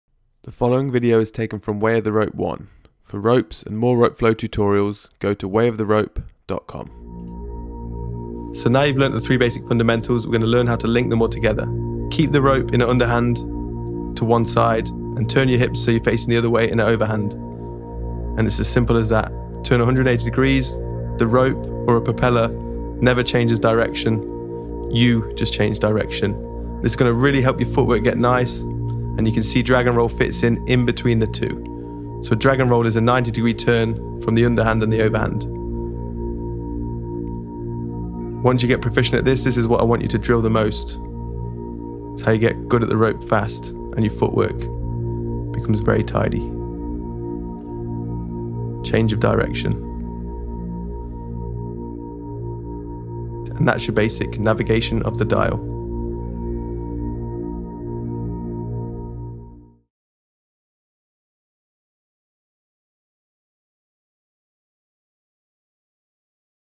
Transitions Between Patterns | ROPE FLOW BEGINNERS
How to transition and circumnavigate between the 3 Primary Patterns of Rope Rolling.

Filmed by Harvey Gibson